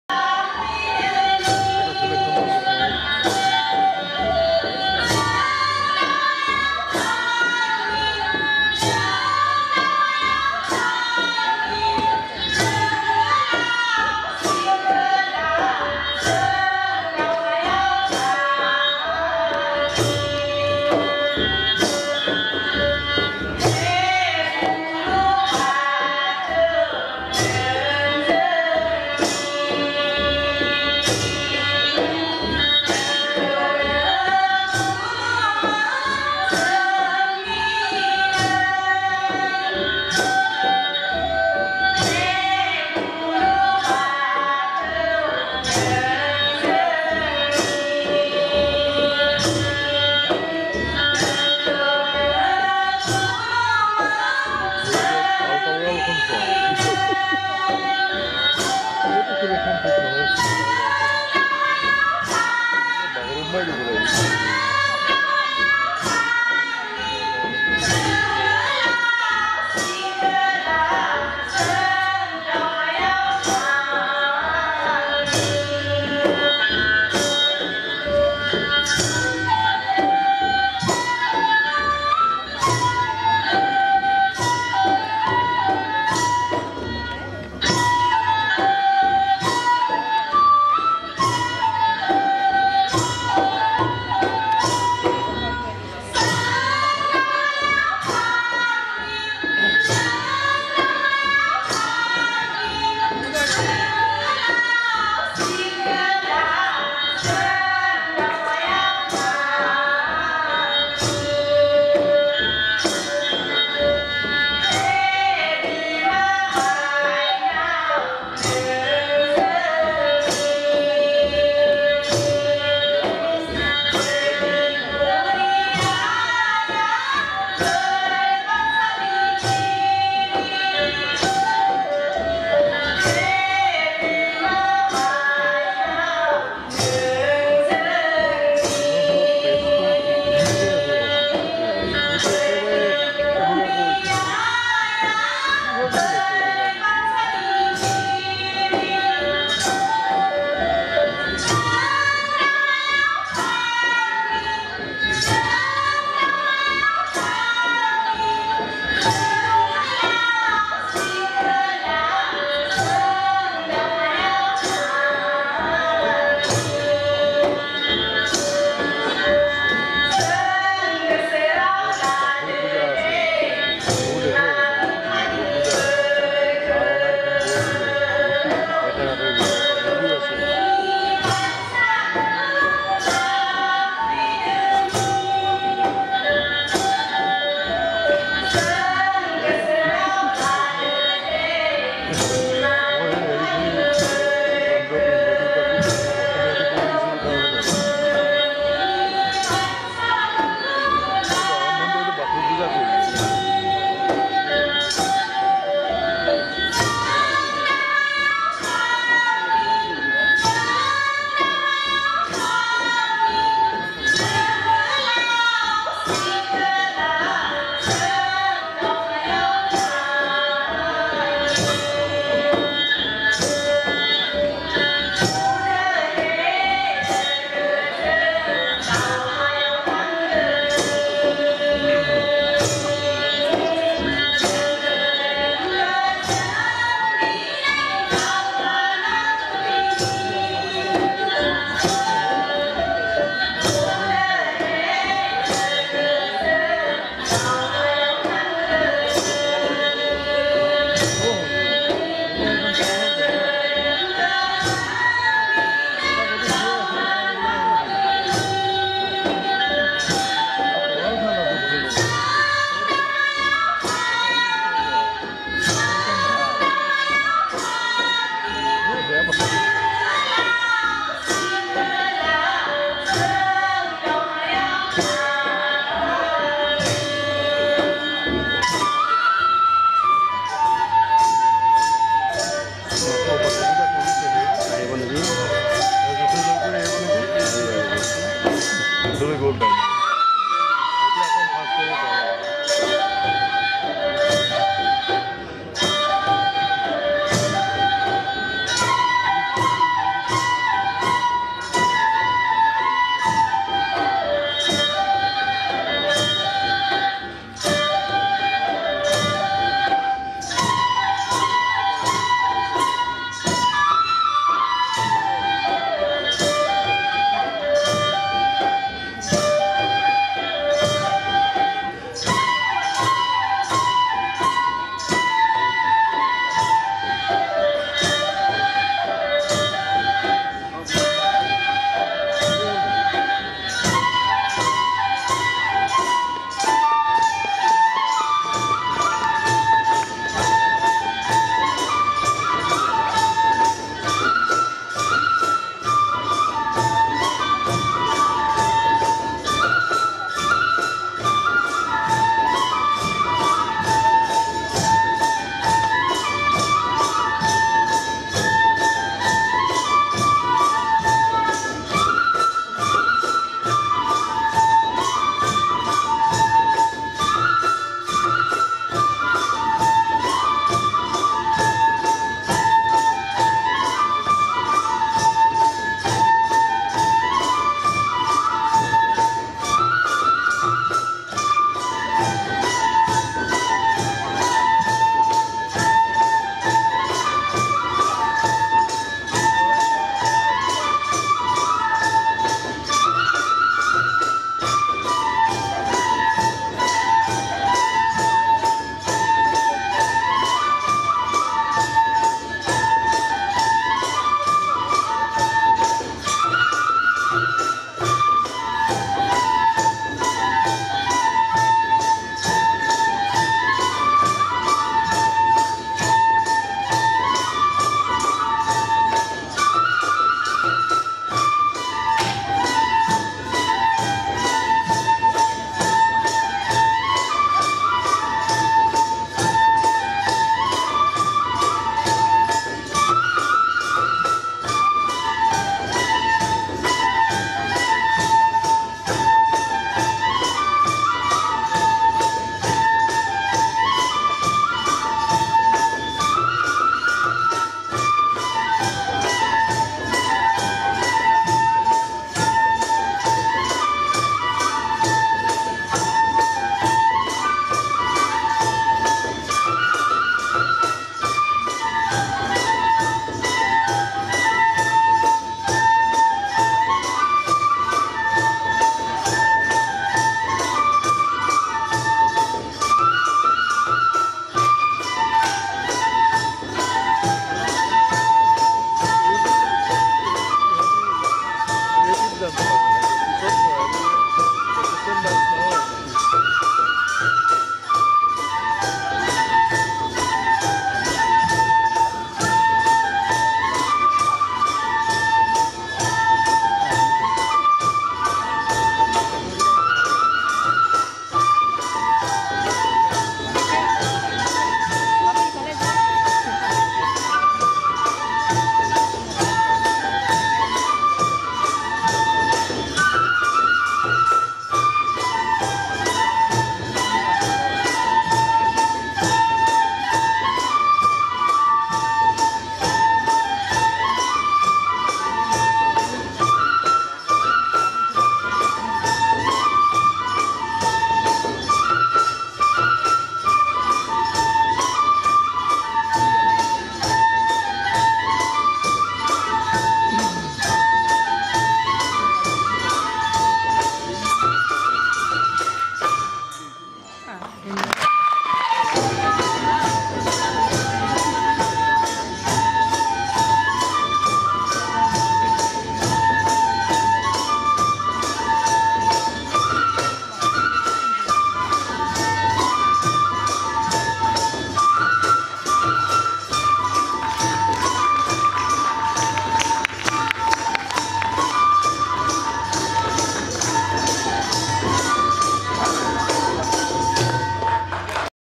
BODO DANCE (Ranachandii Dance)
By Dattaram Boro & Party

AIR FM MAHABAHU Inauguration Program
By All India Radio Guwahati
Srimanta Sankrdev Kalakshetra
8 August 2019